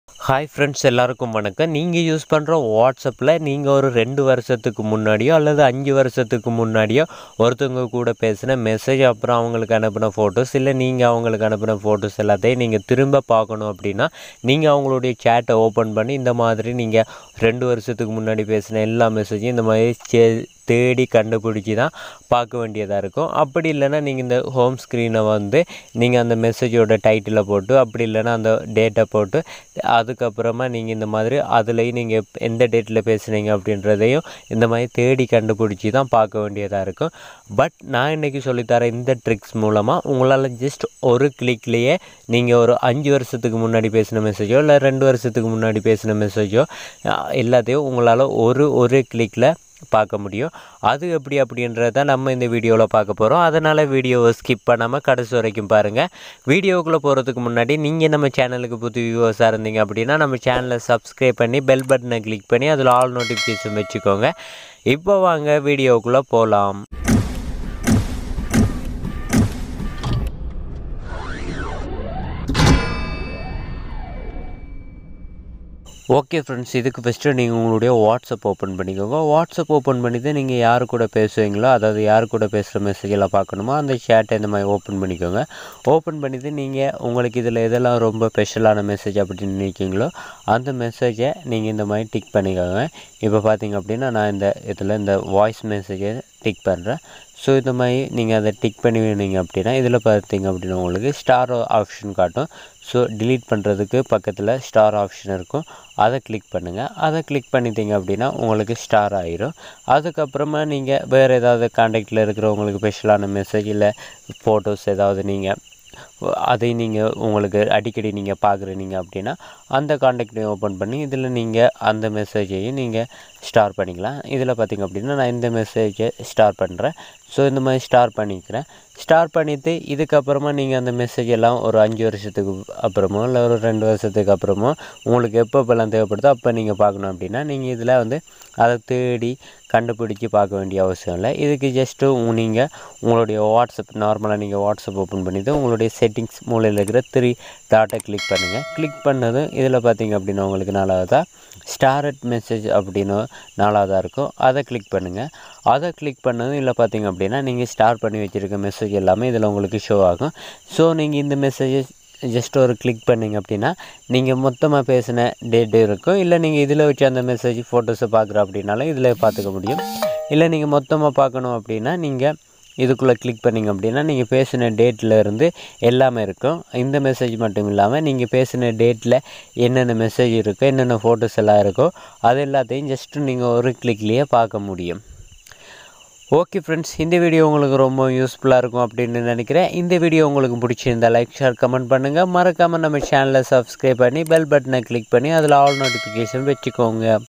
பழைய whatsapp message & photos _ஐ ஈசியாக எடுக்காலாம் / whatsapp tricks and tips in Tamil 2023
this video about WhatsApp secret tricks and tips in Tamil 2023.

thanks for 500subs

thanks for your supporting friends